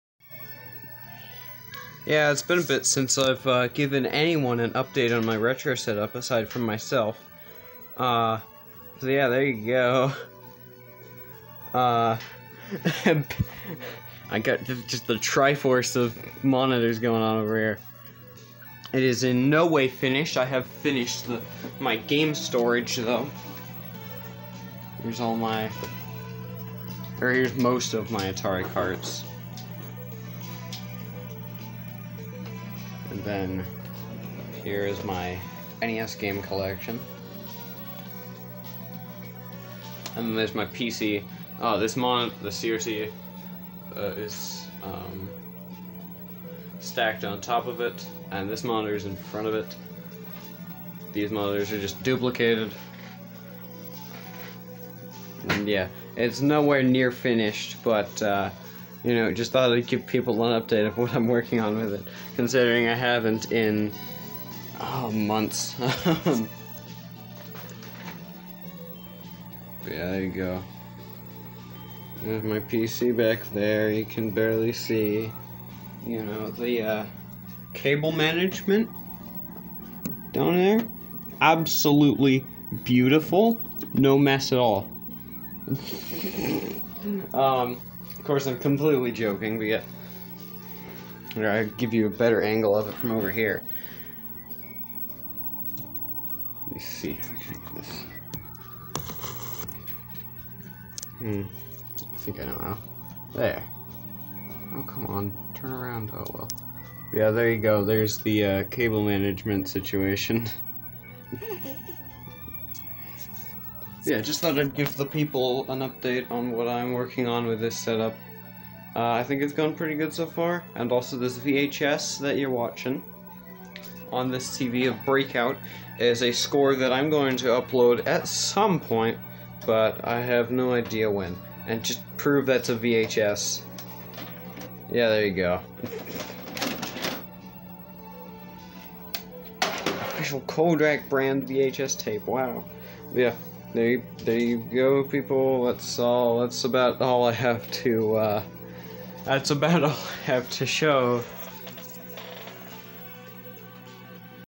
A little update on my Retro Setup!
Yup it certainly has been awhile but here it is my retro setup after several months it isn't finished at all but it is still neat though!